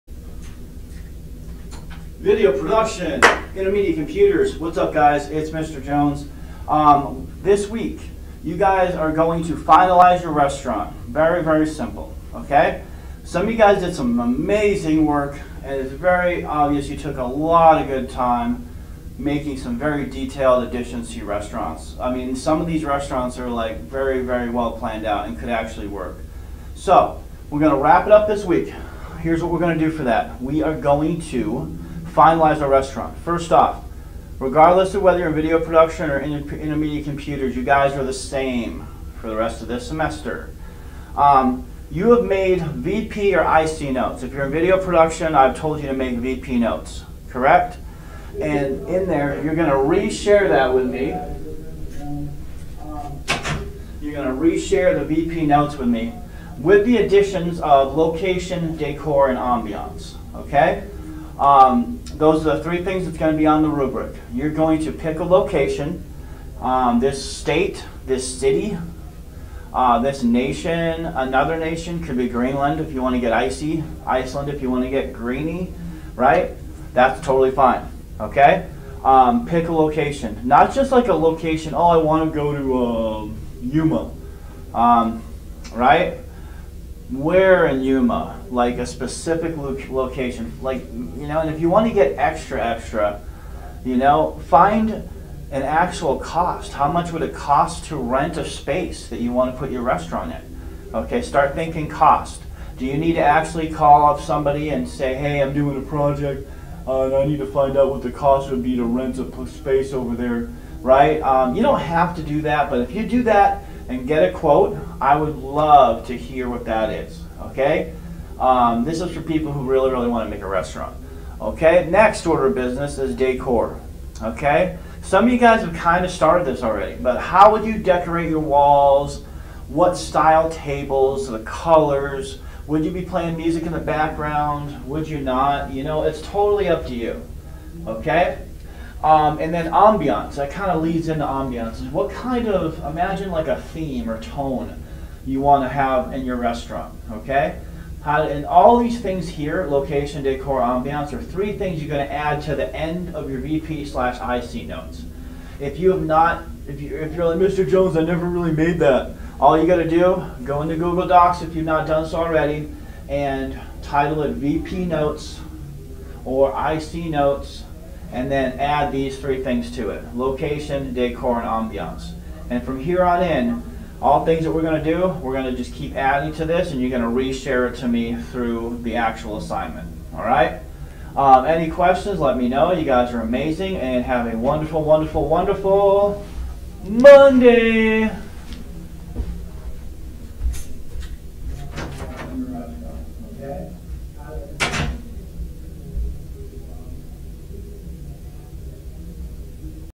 April 20th 2020 Video Production Intermediate Computers Assignment
Pay more attention to the Audio to help out... the video was on the wrong camera... ? Finalize your restaurant and find a Location, Décor, and Ambience for the restaurant... Use Closed Captioning (CC) to help if needed.
-Mr. Jones